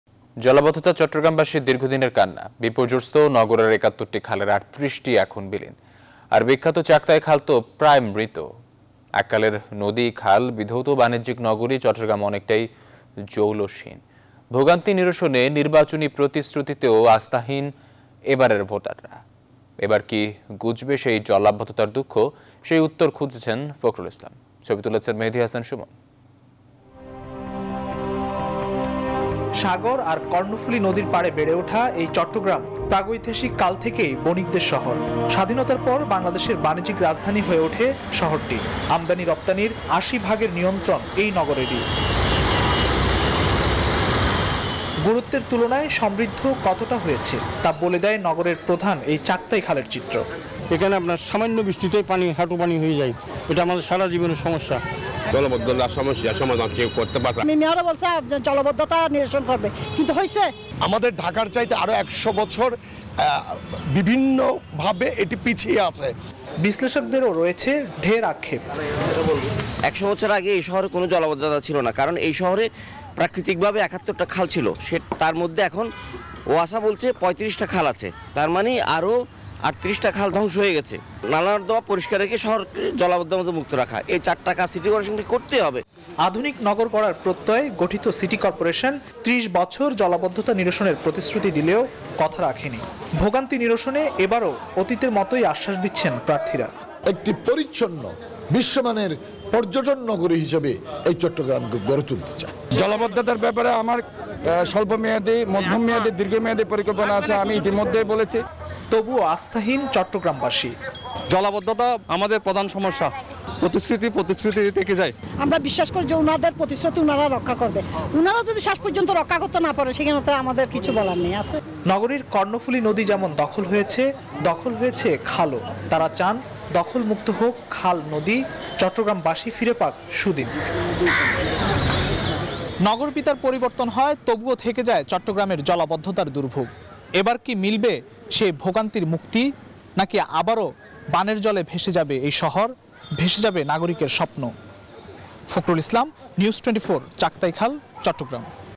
এককালের নদী খাল বিধৌত বানিজ্যিক নগরী চট্টগ্রাম অনেকটাই জৌলুসহীন 22Jan.21
জলাবদ্ধতা চট্টগ্রামবাসীর দীর্ঘদিনের কান্না।বিপর্যস্ত নগরের ৭১ টি খালের ৩৮ টিই এখন বিলীন। আর বিখ্যাত চাক্তাই খাল তো প্রায় মৃত। এককালের নদী খাল বিধৌত বানিজ্যিক নগরী চট্টগ্রাম অনেকটাই জৌলুসহীন। ভোগান্তি নিরসনে নির্বাচনী প্রতিশ্রুতিতেও আস্থাহীন এবারের ভোটাররা। এবার কি ঘুছবে সেই জলাবদ্ধতার দুঃখ? সেই উত্তর খুঁজেছেন ফখরুল ইসলাম। ছবি তুলেছেন মেহেদী হাসান সুমন।

সাগর আর কর্ণফুলী নদীর পাড়ে বেড়ে ওঠা এই চট্টগ্রাম প্রগৈতিহাসিক কাল থেকেই বনিকদের শহর। স্বাধীনতার পর বাংলাদেশের বানিজ্যিক রাজধানী হয়ে ওঠে শহরটি। আমদানী রপ্তানীর ৮০ ভাগের নিয়ন্ত্রণ এ নগরেরই।

গুরুত্বের তুলনায় সমৃদ্ধ কতটা হয়েছে? তা বলে দেয় নগরের প্রধান এই চাক্তাই খালের চিত্র।

বিশ্লেষকদেরও রয়েছে ঢের আক্ষেপ।

আধুনিক নগর গড়ার প্রত্যয়ে গঠিত সিটি কর্পোরেশন ৩০ বছর জলাবদ্ধতা নিরসনের প্রতিশ্রুতি দিলেও, কথা রাখেনি। ভোগান্তি নিরসনে এবারও অতীতের মতোই আশ্বাস দিচ্ছেন প্রার্থীরা।

তবুও আস্থাহীন চট্টগ্রামবাসী। নগরীর কর্ণফূলী নদী যেমন দখল হয়েছে দখল হয়েছে খালও। তারা চান দখলমুক্ত হোক খাল নদী। চট্টগ্রামবাসী ফিরে পাক সুদিন।

On Aired on NEWS24 on 22nd January, 2021
Our other YouTube channels :
News24 Official Address:
NEWS24
371/A
Bashundhara Road
Block – D
Bashundhara Residential Area
Dhaka - 1229